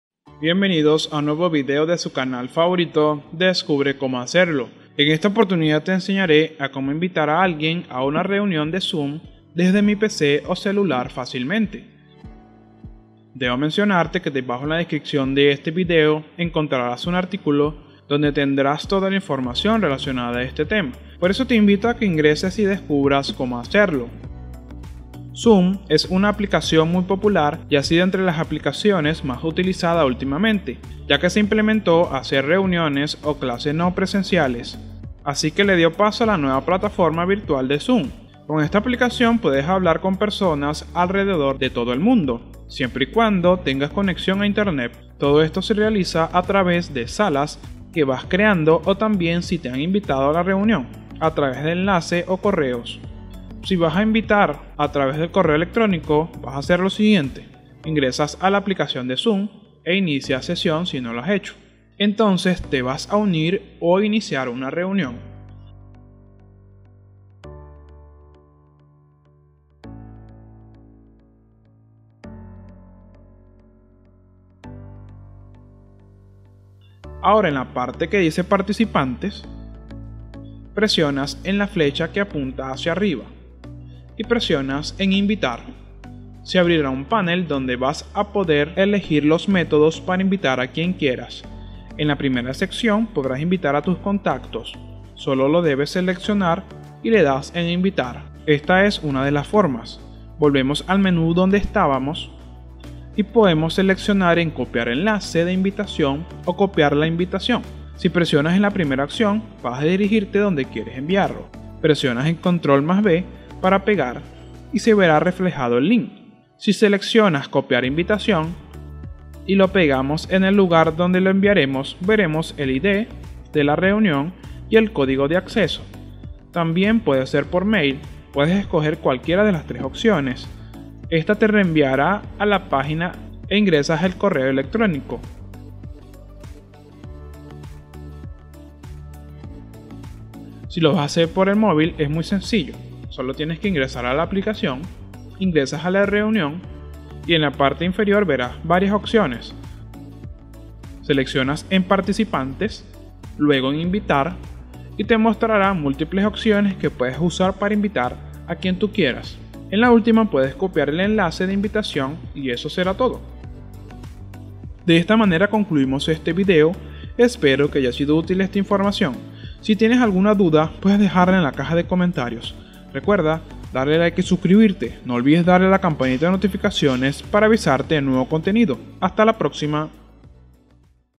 ¿Cómo invitar a alguien a una reunión de ZOOM desde mi PC o celular fácilmente?
En este video descubre Cómo invitar a alguien a una reunión de ZOOM desde mi PC o celular fácilmente. En el Canal Descubre Cómo Hacerlo encontrarás el mejor contenido. ¡No te lo puedes perder!